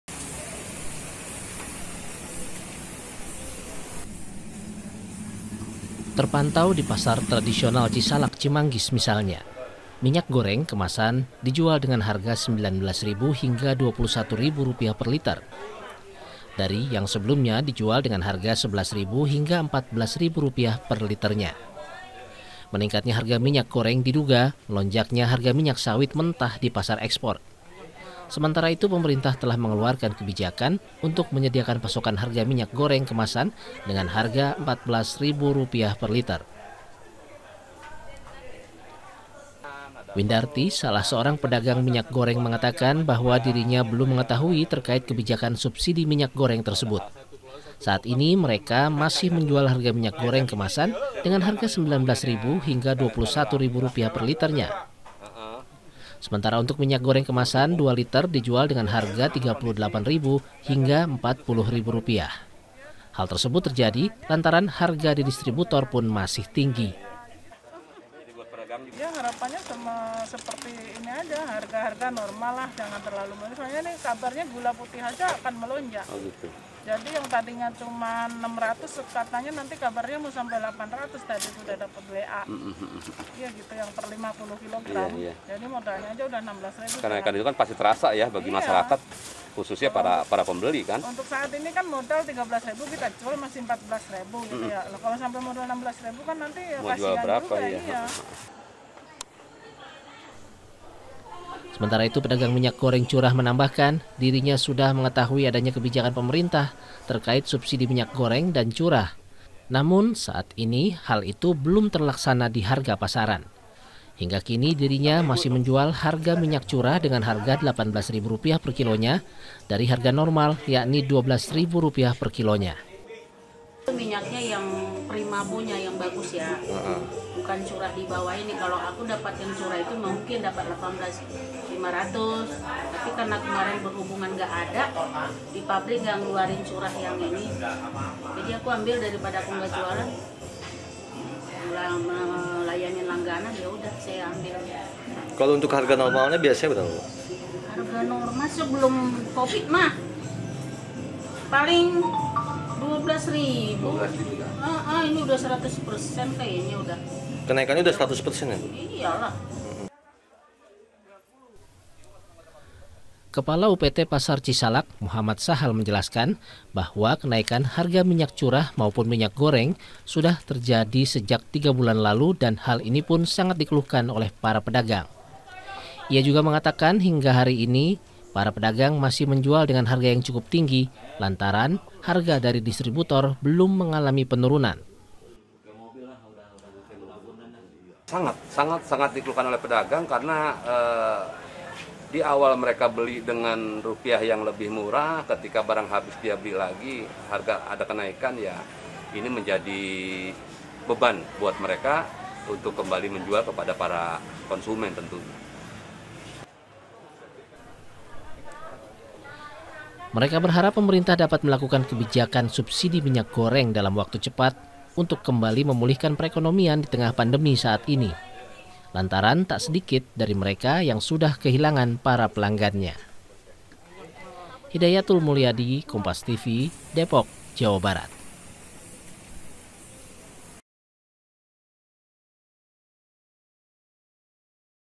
Pedagang Mengeluh Karena Harga Minyak Goreng Masih Tinggi
DEPOK, KOMPAS.TV - Terpantau di Pasar Tradisional Cisalak Cimanggis misalnya minyak goreng kemasan dijual dengan harga 19 ribu hingga 21 ribu rupiah perliternya

Dari yang sebelumnya dijual dengan harga 11 hingga 14 ribu rupiahnya perliternya

Meningkatnya harga minyak goreng diduga melonjaknya harga minyak sawit mentah di pasar ekspor sementara itu pemerintah telah mengeluarkan kebijakan untuk menyediakan pasokan harga minyak goreng kemasan dengan harga 14 ribu rupiah perliter

Windarti seorang pedagang minyak goreng mengatakan bahwa dirinya belum mengetahui terkait kebijakan subsidi minyak goreng tersebut saat ini mereka masih menjual harga minyak goreng kemasan dengan harga 19 ribu hingga 21 ribu perliternya sementara untuk minyak goreng kemasan dua liter dijual dengan harga 38 ribu hingga 40 ribu rupiah hal tersebut terjadi lantaran harga dari distributor pun masih tinggi

Sementara itu pedagang minyak goreng curah menambahkan dirinya sudah mengetahui adanya kebijakan pemerintah terkait subsidi minyak goreng dan curah namun saat ini hal itu belum terlaksana di harga pasaran hingga kini dirinya masih menjual harga minyak curah dengan harga 18 ribu rupiah perkilonya dari harga normal yakni 12 ribu rupiah perkilonya

Kepala UPT Pasar Cisalak Muhammad Sahal menjelaskan bahwa kenaikan harga minyak curah maupun minyak goreng sudah terjadi sejak tiga bulan lalu dan hal inipun sangat dikeluhkan oleh para pedagang ia juga mengatakan hingga hari ini pedagang masih menjual dengan harga yang cukup tinggi lantaran harga dari distributor belum mengalami penurunan

Mereka berharap pemerintah dapat melakukan kebijakan subsidi minyak goreng dalam waktu cepat untuk kembali memulihkan perekonomian ditengah pandemi saat ini lantaran tak sedikit dari mereka sudah kehilangan para pelanggannya

Untuk lebih tahu berita terupdate seputarJawa Barat, bisa klik link di bawah .